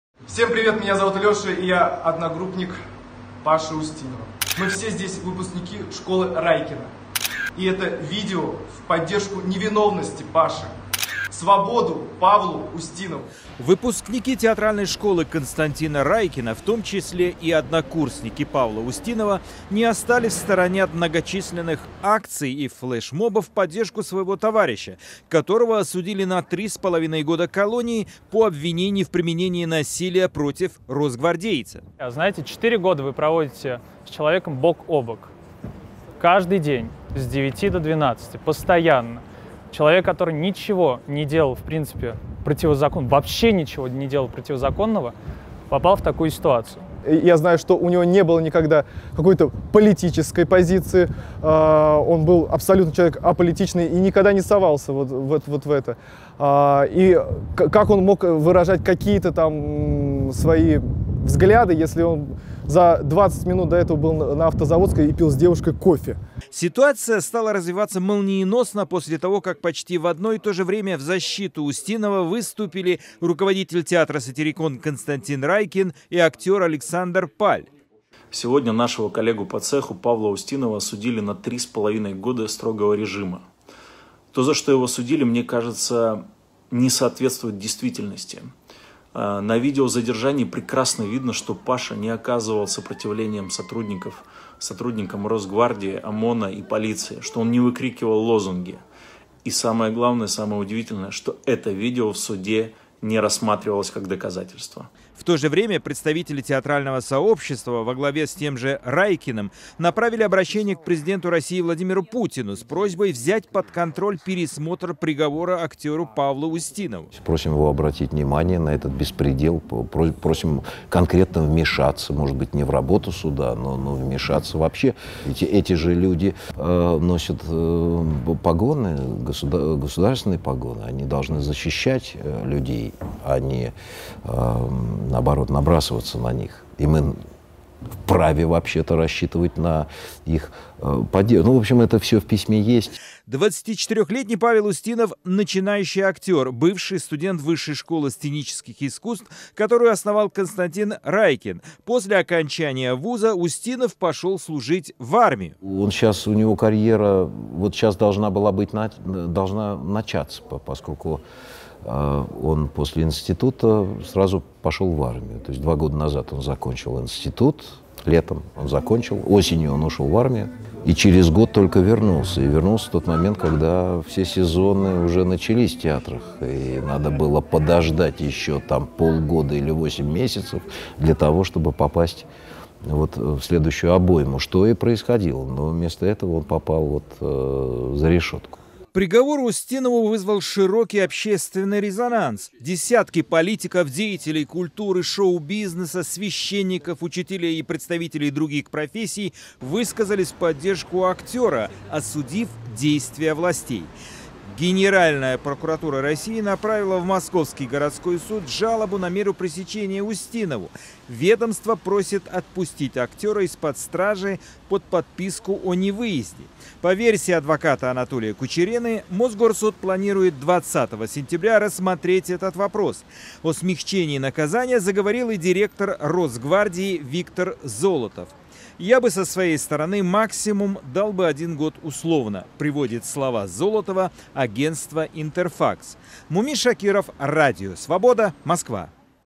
"Вместо театра попал за решетку"
Выпускники театральной школы Константина Райкина выступили в поддержку своего коллеги Павла Устинова. Актер был осужден на три с половиной года колонии по обвинению в применении насилия против росгвардейца.